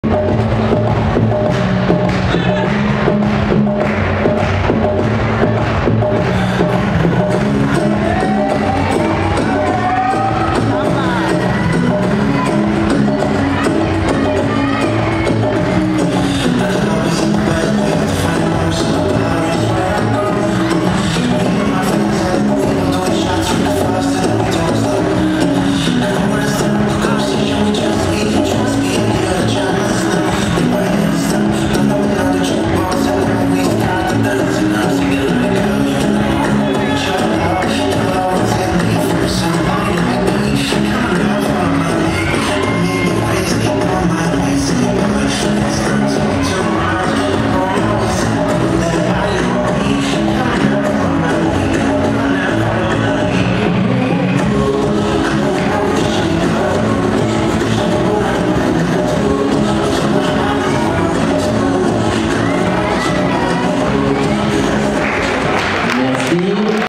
Baptiste DECRULLE / Alexane PIRO - Ch. Régional Occ/PACA/Corse 2023, Juvéniles 1 Latine, (Samba)
Championnat Régional Occitanie, PACA, Corse
Narbonne, France
11/02/2023

Catégorie Juvéniles 1 Latine :
Classement final :
1. Baptiste DECRULLE / Alexane PIRO
2. Juliana FROUX / Chloé CORREIA